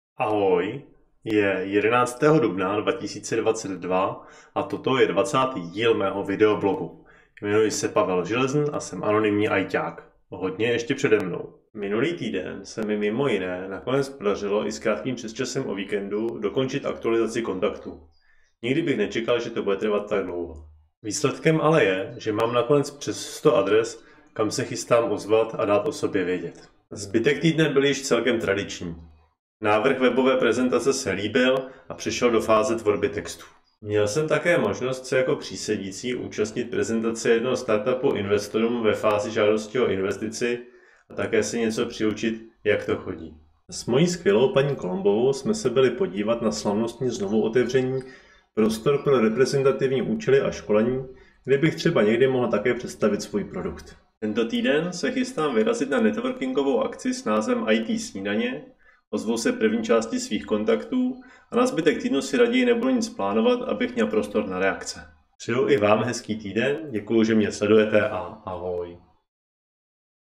2022-04-11 - Kontakty aktualizovány
Několik dní přepínání mezi kontakty, linkedinem,  facebookem a několika osobními prezentacemi je za mnou, ale stálo to za to. Těším se co z toho vznikne